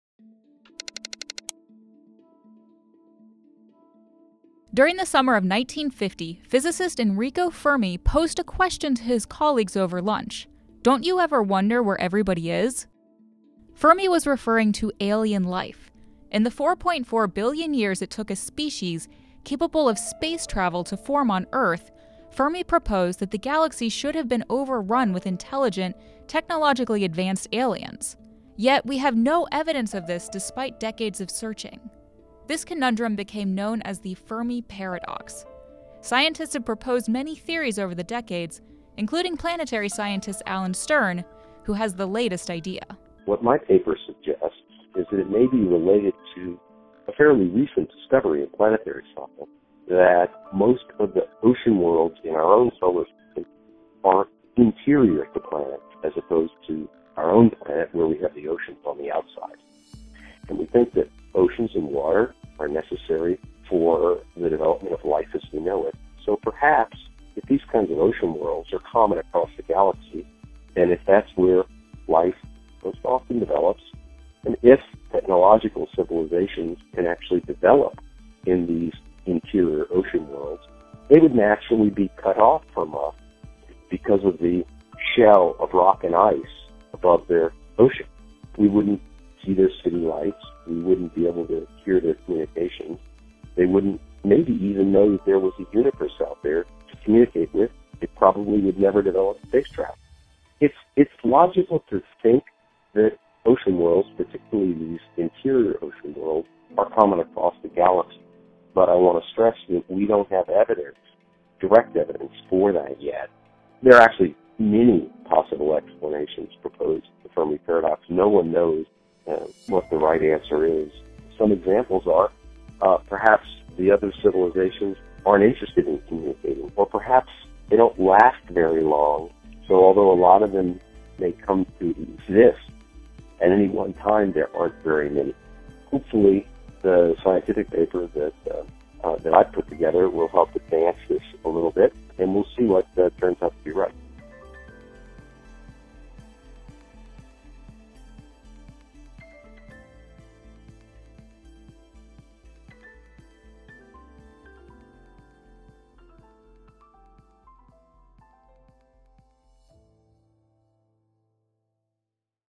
Why Haven't We Heard From Aliens Yet?
During the summer of 1950, physicist Enrico Ferm posed a question to his colleagues over lunch, "Don't you ever wonder where everybody is?" Fermi was referring to alien life. In the 4.4 billion years it took a species capable of space travel to form on Earth, Fermi proposed that the galaxy should have been overrun with intelligent, technologically-advanced aliens. Yet, we have no evidence of this despite decades of searching. This conundrum became known as the Fermi Paradox. Scientists have proposed many theories over the decades, including planetary scientist, Alan Stern, who has the latest idea.

Alan Stern: What my paper suggests is that it may be related to a fairly recent discovery of planetary science that most of the ocean worlds in our own solar system are interior to planets as opposed to our own planet where we have the oceans on the outside. And we think that oceans and water are necessary for the development of life as we know it, so perhaps, if these kinds of ocean worlds are common across the galaxy, and if that's where life most often develops. And if technological civilizations can actually develop in these interior ocean worlds, they would naturally be cut off from us because of the shell of rock and ice above their ocean. We wouldn't see their city lights. We wouldn't be able to hear their communication. They wouldn't maybe even know that there was a universe out there to communicate with. They probably would never develop space travel.

It's logical to think that ocean worlds, particularly these interior ocean worlds, are common across the galaxy. But I want to stress that we don't have evidence, direct evidence for that yet. There are actually many possible explanations proposed for Fermi Paradox. No one knows what the right answer is. Some examples are, perhaps the other civilizations aren't interested in communicating or perhaps they don't last very long, so although a lot of them, they come to exist, at any one time, there aren't very many. Hopefully, the scientific paper that I put together will help advance this a little bit and we'll see what turns out to be right.